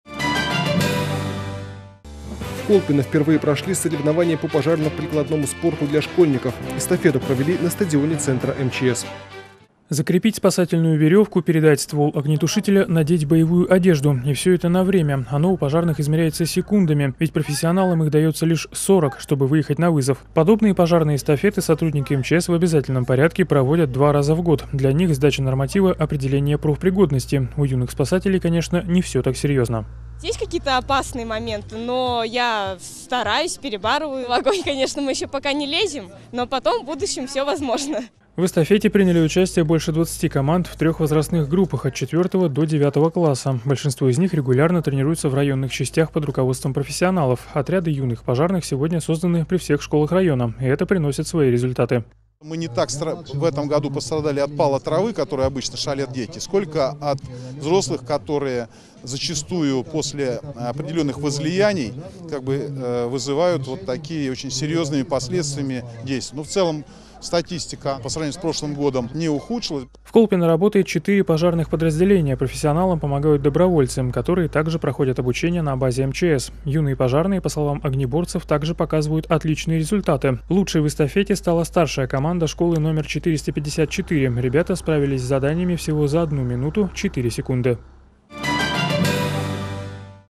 Школьники померились силами в тушении огня и спасении людей.
В Колпино впервые прошли соревнования по пожарно-прикладному спорту.
tkt-tv.tv